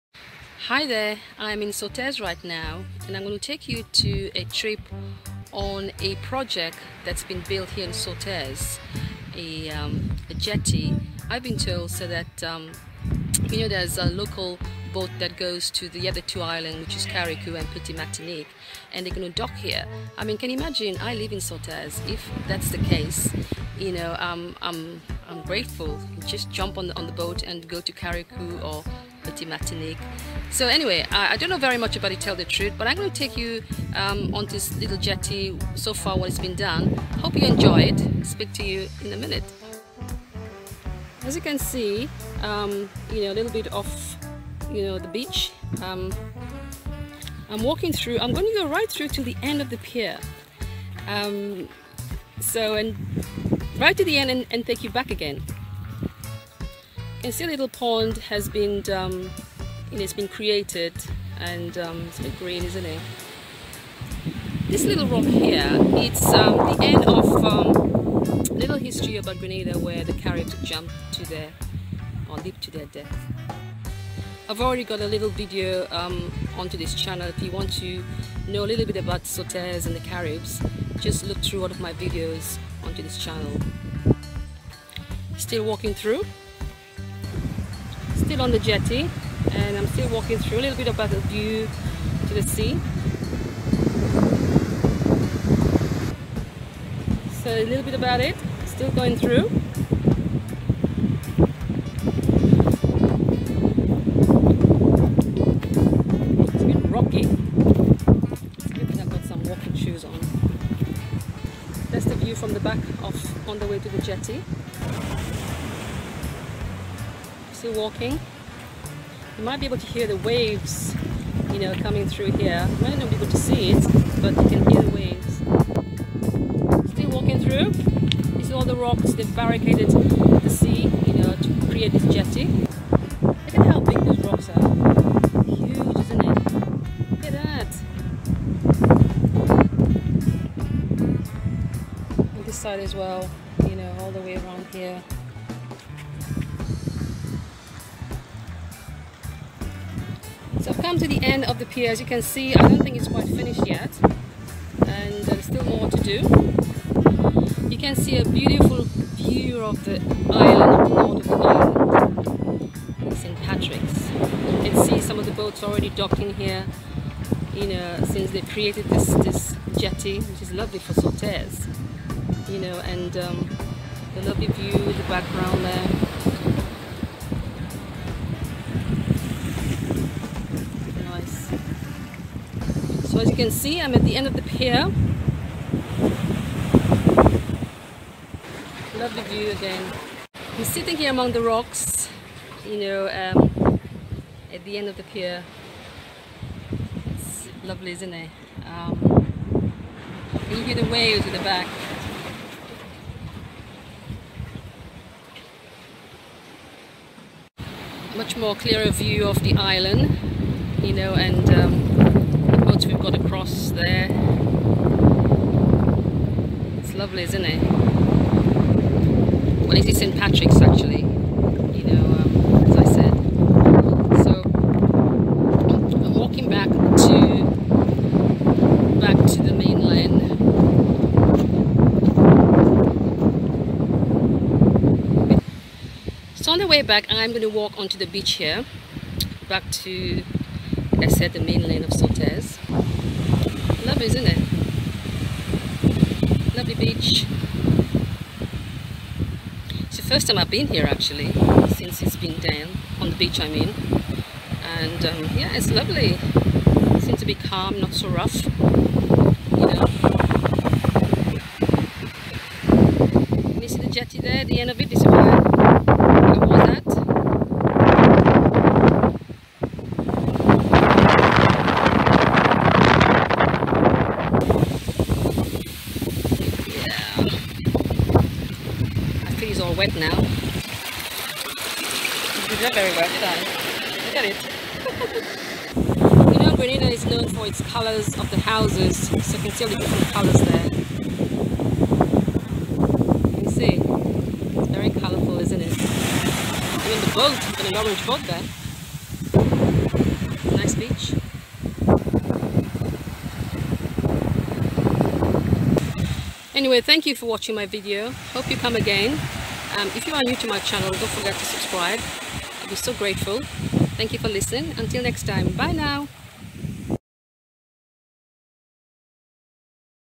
Grenada | Long walk on a short Pier | 2020
A Jetty or long beach Pier project in Sauteurs. A nice view of the island in St Patricks, Grenada Topic, Caribbean, Northern part of the Island.